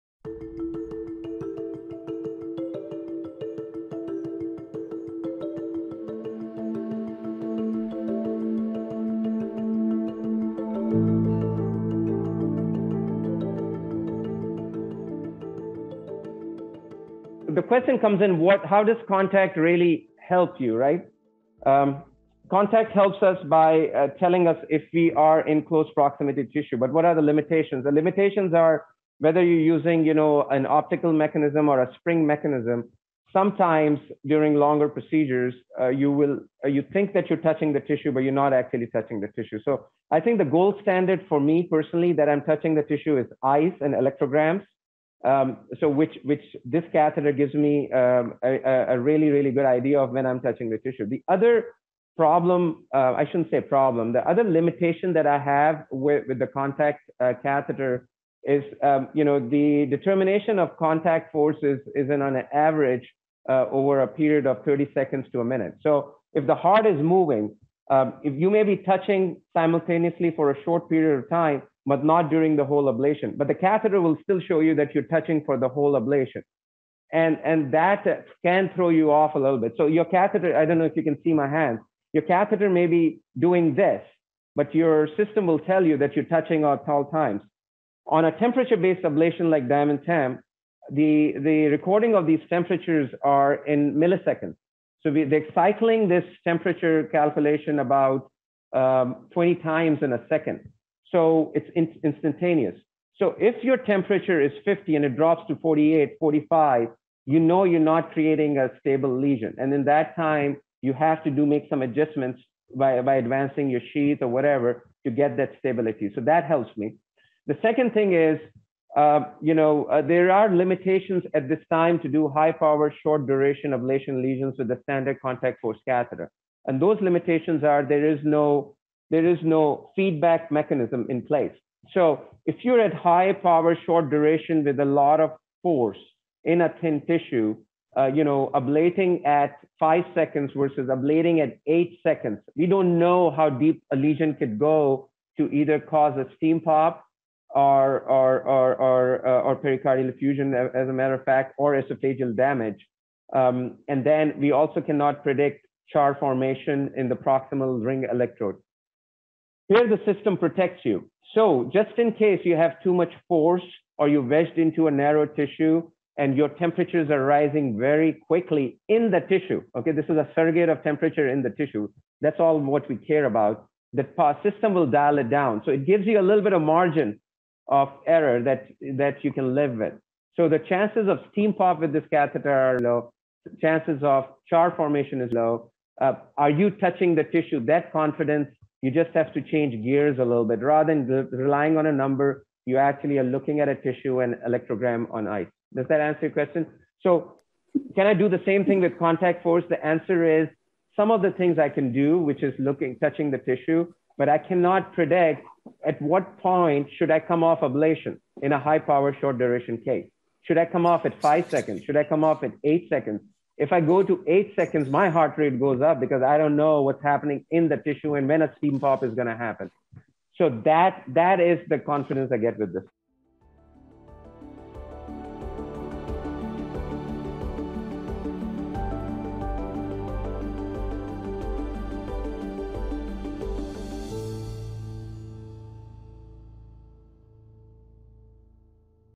Contact force assessment with the DiamondTemp™ ablation catheter
Watch Dr. Usman Siddiqui, M.D., FHRS share his assessment on contact force using the DiamondTemp™ ablation catheter.